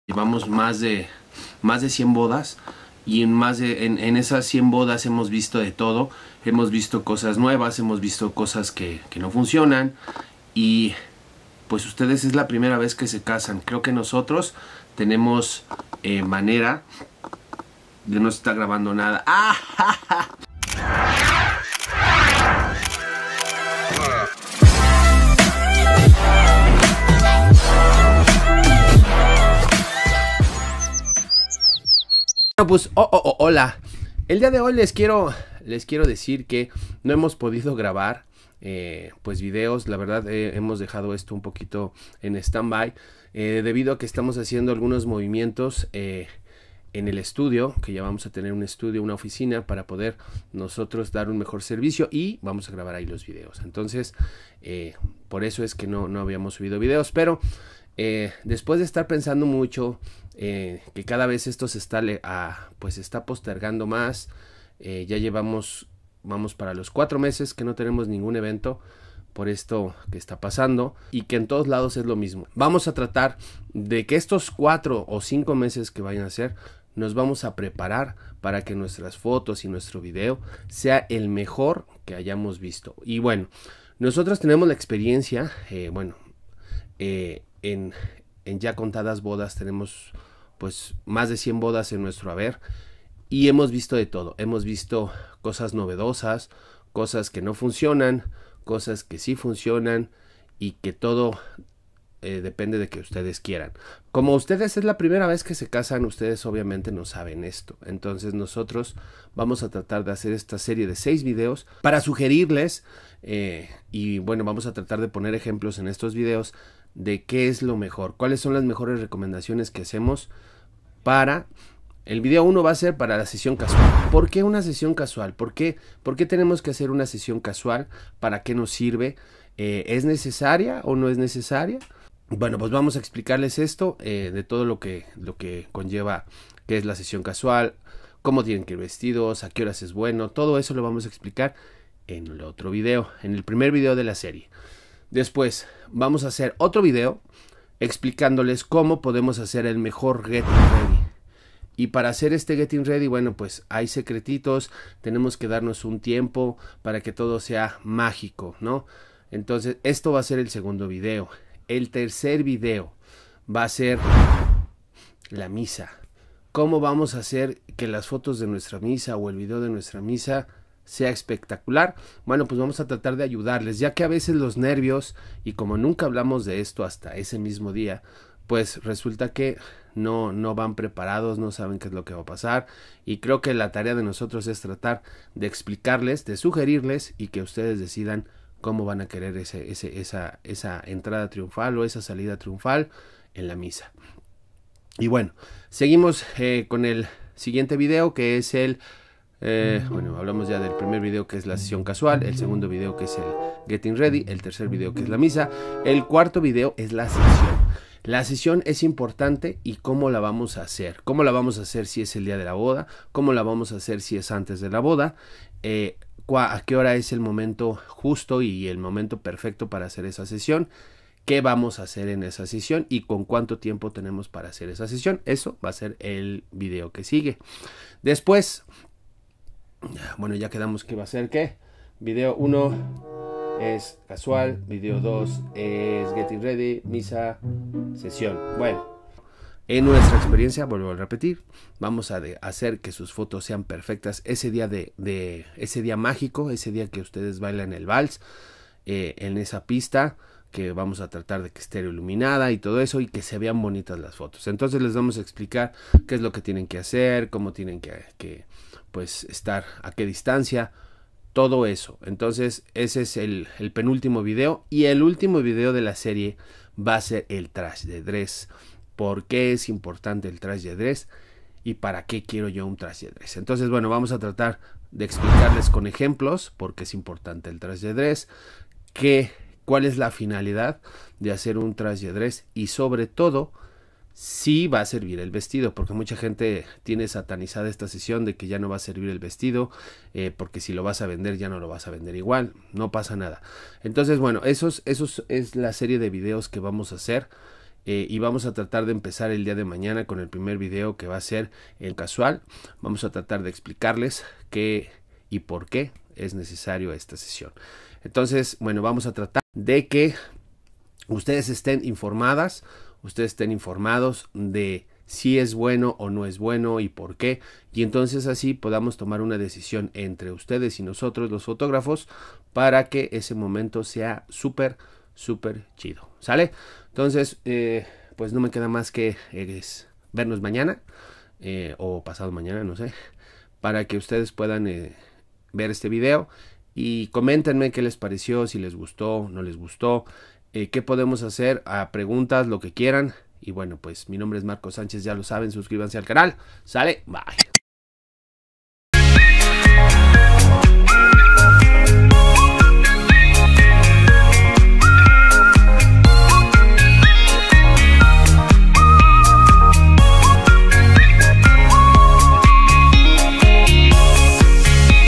Que hacer para que tu Boda sea PERFECTA😱
Les cuento como será la serie de 6 videos donde les daremos algunos consejos para que su boda sea PERFECTA en cuestión de foto y video,  te invito a que te suscribas y juntos aprendamos.

Grupo de WhatsApp para participar en las sesiones gratis que regalamos a nuestros suscriptores